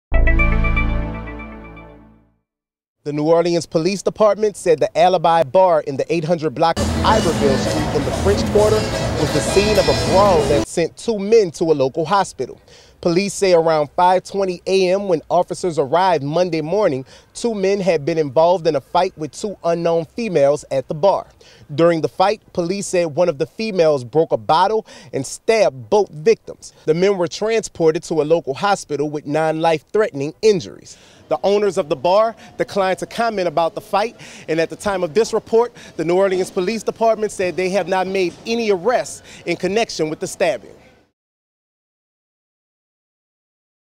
French Quarter bar brawl leaves two men stabbed
The New Orleans Police Department is investigating a incident in the 800 block of Iberville Street that sent two men to a local hospital with cuts after a bar brawl with two women.